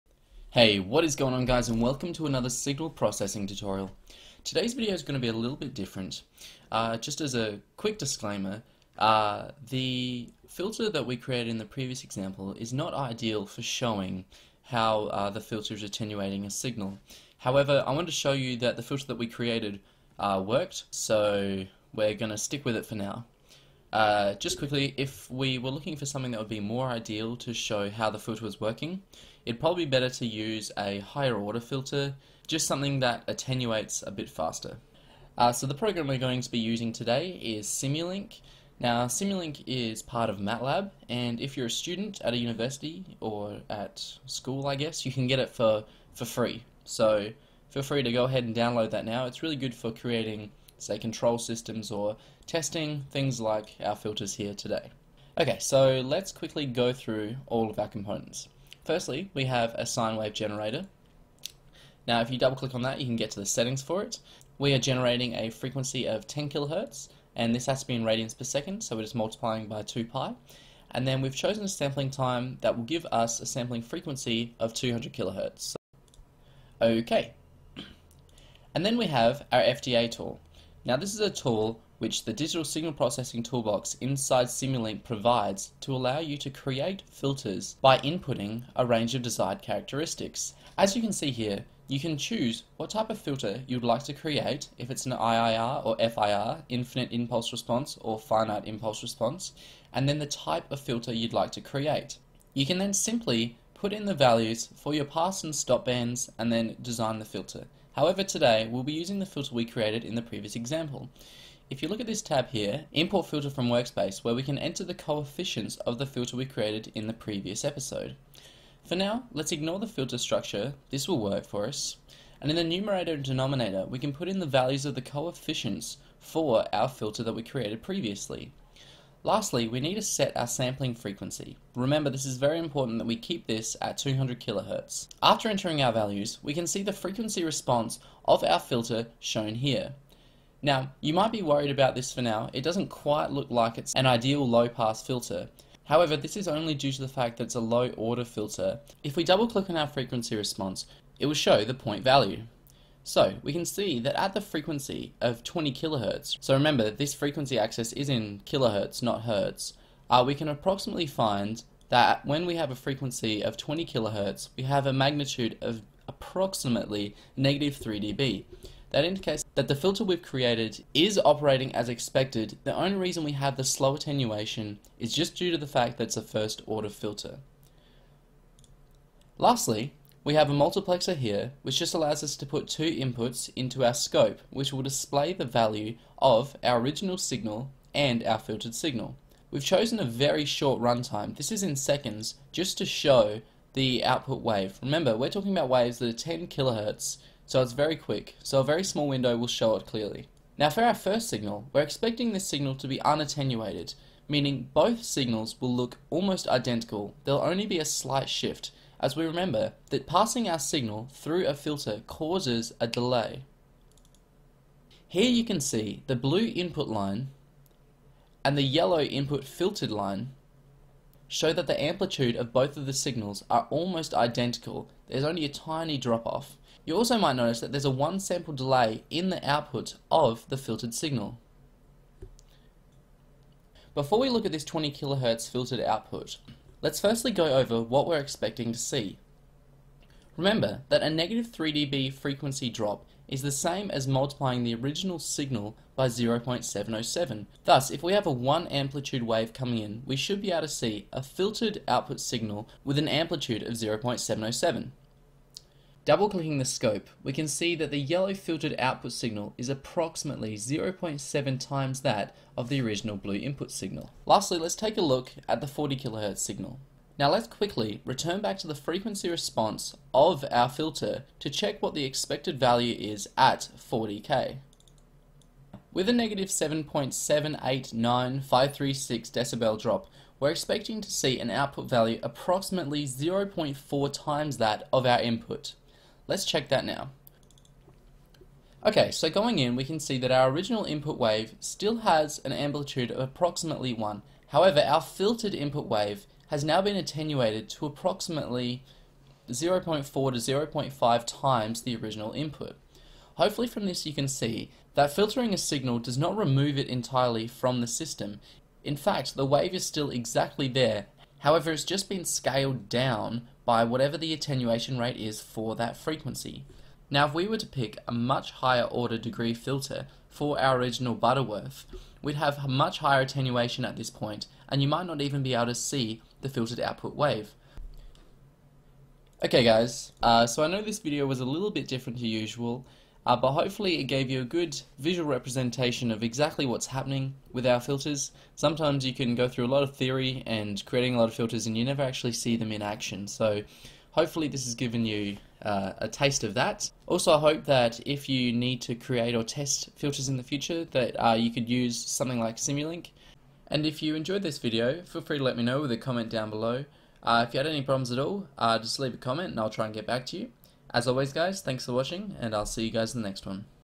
(How to) Create a Digital Filter in MATLAB Simulink - Signal Processing #17
An implementation of the filter we created in our previous video using MATLAB and Simulink